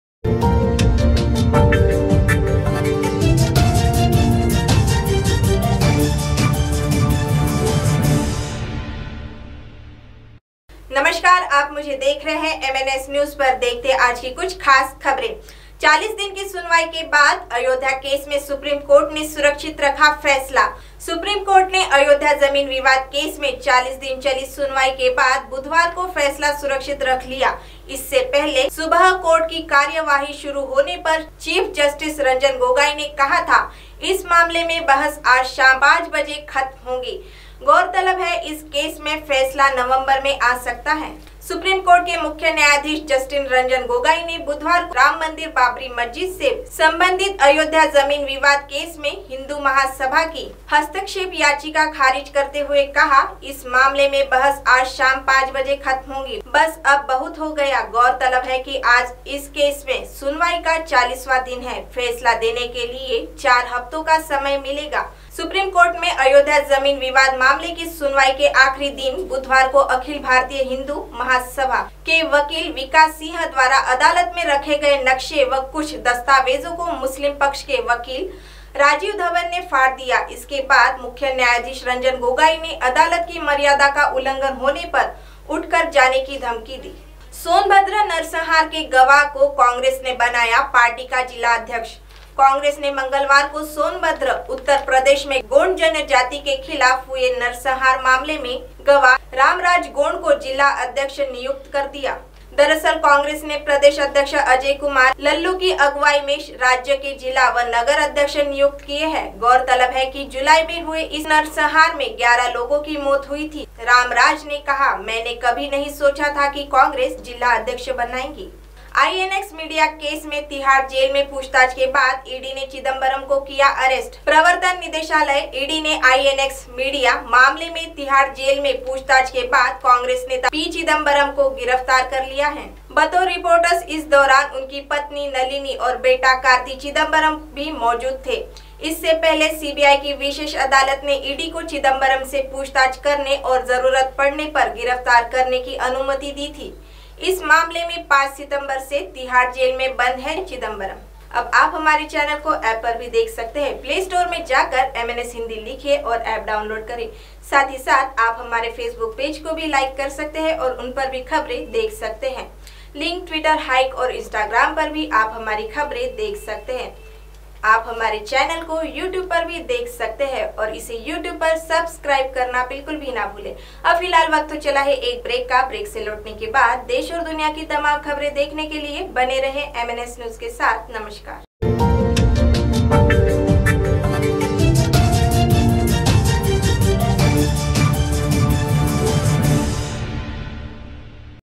MNS #40 दिन की सुनवाई के बाद अयोध्या केस में सुप्रीम कोर्ट ने सुरक्षित रखा फैसला।
MNSNEWSLIVE #40 दिन की सुनवाई के बाद अयोध्या केस में सुप्रीम कोर्ट ने सुरक्षित रखा फैसला।